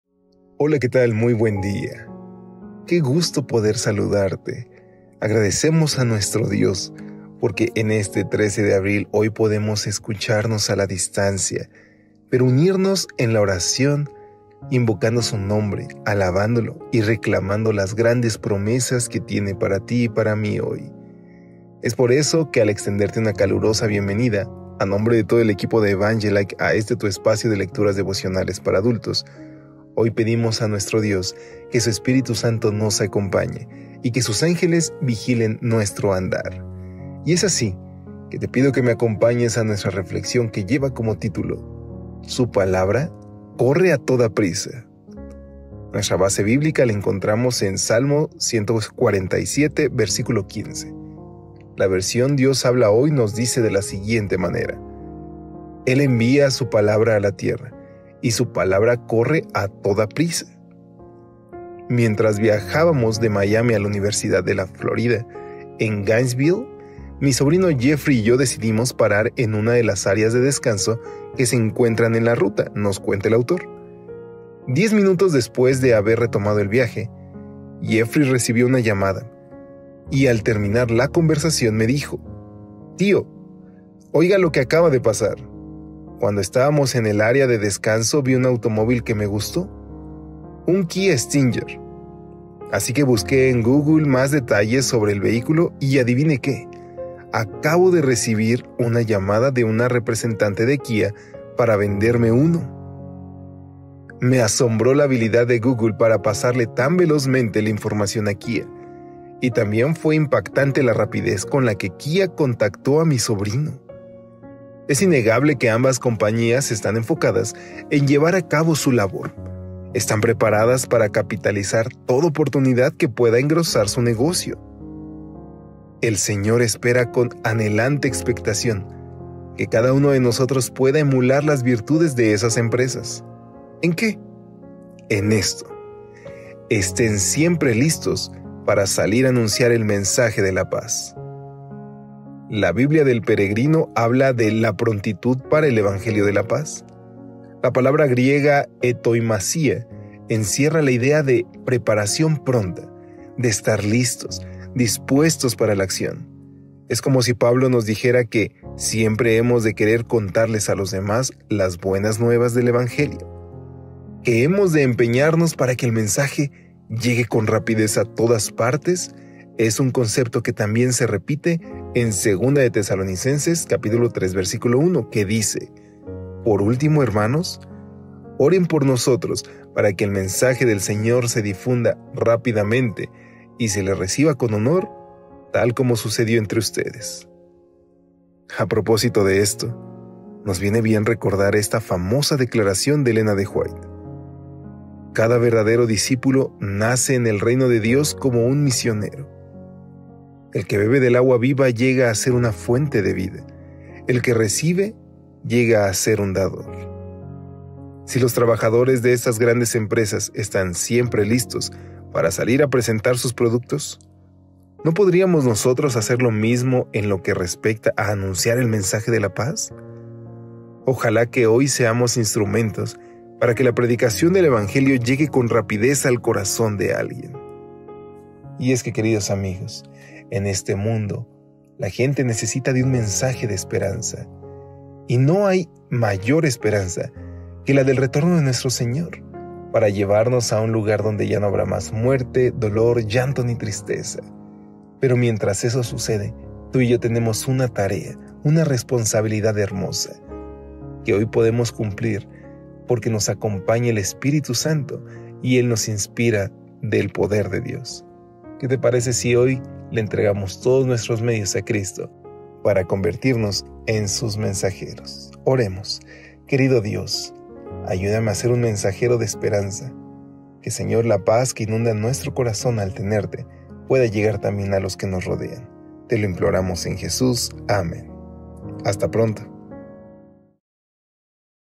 Devocional Cristiano para Adultos | 13 de abril de 2023
Devocional Cristiano para Adultos - YO ESTOY CONTIGO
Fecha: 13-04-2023
Título: SU PALABRA CORRE A TODA PRISA
Autor: Vladimir Polanco
Locución: Elías Gómez